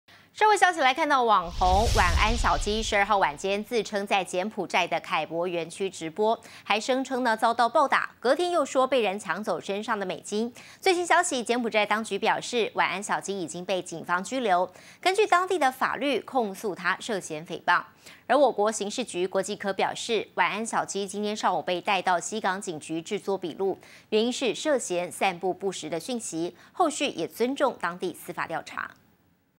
網紅「晚安小雞」慘了 涉嫌誹謗遭柬埔寨警逮捕【最新快訊】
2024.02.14【最新快訊】網紅「晚安小雞」12號晚間自稱在柬埔寨的凱博園區直播，還聲稱遭到暴打，隔天又說被人搶走身上的美金。最新消息柬埔寨當局表示，「晚安小雞」已經被警方拘留，根據當地法律控訴他涉嫌誹謗。我國刑事局國際科表示，「晚安小雞」今（14）日上午被帶到西港警局製作筆錄，原因是涉嫌散佈不實訊息，後續也尊重當地司法調查。